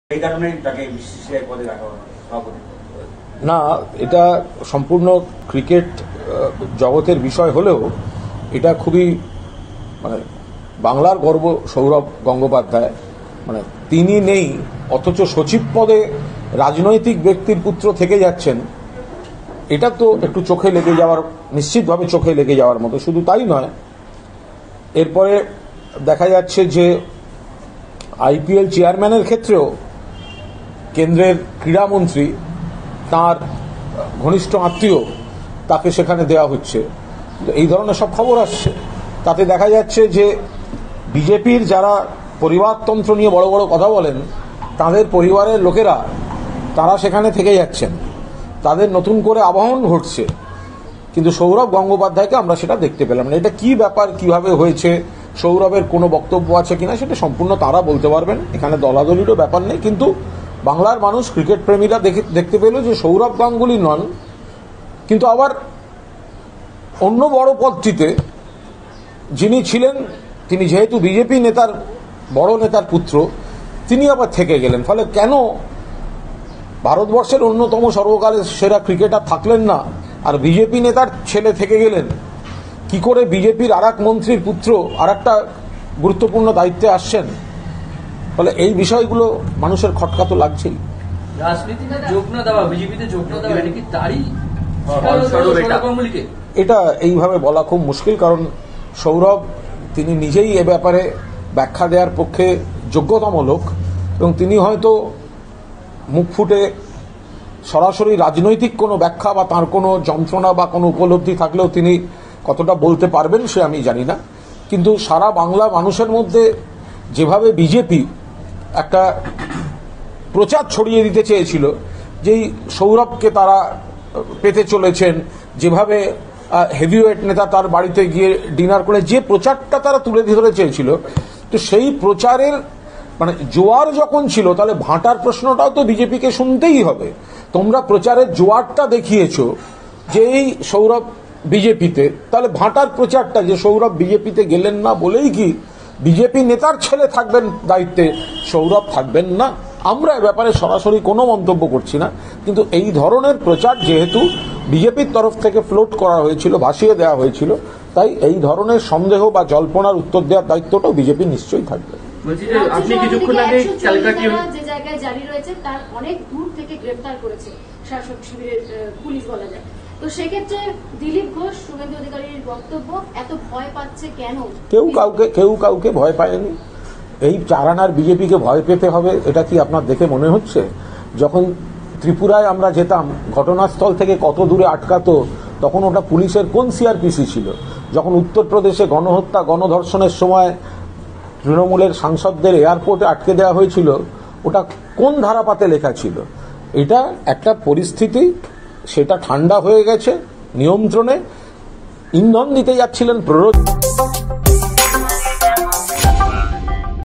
সৌরভের BCCI প্রেসিডেন্ট পদ থেকে সরানোর জল্পনায় বিজেপিকে নিশানা কুণালের
কলকাতা টুডে ব্যুরো: মঙ্গলবার BCCI-এর বিভিন্ন পদগুলির দায়িত্বে আগামী দিনে কারা থাকছেন, সেই নিযে বৈঠক সারলেন ভারতীয় ক্রিকেট বোর্ড।  সূত্রের খবর, বোর্ড প্রেসিডেন্টের পদে আর থাকছেন না সৌরভ গঙ্গোপাধ্যায়। সেই জায়গায় আনা হতে পারে রজার বিনিকে। তবে সৌরভ প্রেডিসেন্ট পদে না থাকলেও বোর্ডের সচিব পদে অমিত শাহর পুত্র জয় শাহই থেকে যাচ্ছেন বলে সূত্রের খবর। আর এই নিয়েই জোর চর্চা শুরু হয়েছে রাজ্য রাজনীতির মহলে। অতীতে সৌরভ গঙ্গোপাধ্যায় বিজেপিতে যোগ দিতে পারেন বলেও গুঞ্জন ছড়িয়েছিল। কিন্তু ‘মহারাজ’ এখনও সেইদিকে এগোননি। সেই কারণেই কি বোর্ড প্রেসিডেন্টের পদ খোয়াতে হচ্ছে সৌরভকে? এমন জল্পনা উস্কে দিয়েছেন তৃণমূল মুখপাত্র কুণাল ঘোষ।

কুণাল ঘোষ মঙ্গলবার সাংবাদিক বৈঠকে বলেন, “এই বিষয়ে ব্যাখ্যা দেওয়ার জন্য যোগ্যতম লোক সৌরভই। তাঁর কোনও রাজনৈতিক ব্যাখ্যা বা উপলব্ধি থাকলেও তিনি কতটা তা মুখ ফুটে বলতে পারবেন, তা আমি জানি না। কিন্তু যেভাবে বিজেপি সারা বাংলার মানুষের মধ্যে একটি প্রচার ছড়িয়ে দিতে চেয়েছিল, সৌরভকে তারা পেতে চলেছে, যেভাবে হেভিওয়েট নেতা তাঁর বাড়িতে গিয়ে নৈশভোজ করেছিলেন। সেই প্রচারের জোয়ার যখন ছিল, তখন ভাটার প্রশ্নটাও তো বিজেপিকে শুনতেই হবে। সৌরভ বিজেপিতে গেলেন না বলেই কি বিজেপি নেতার ছেলে থাকবেন দায়িত্বে আর সৌরভ থাকবেন না?” সঙ্গে অবশ্য কুণাল ঘোষ আরও বলেন, “আমরা এই বিষয়ে সরাসরি কোনও মন্তব্য করছি না। কিন্তু এই ধরনের প্রচার যেহেতু বিজেপির তরফ থেকে ভাসিয়ে দেওয়া হয়েছিল, তাই এই ধরনের জল্পনার জবাব দেওয়ার দায়িত্বও বিজেপির নিশ্চয়ই থাকবে।”